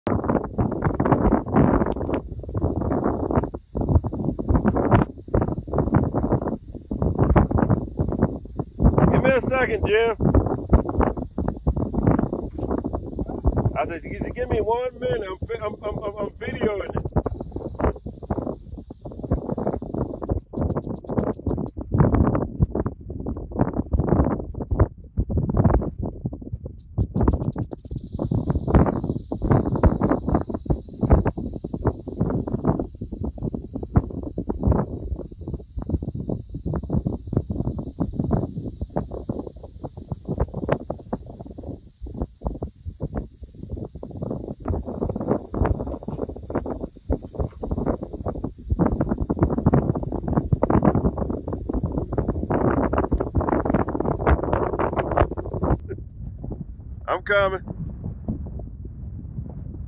Rattlesnake warning
This rattlesnake was spotted in Rio Grande Estates off Hwy 60.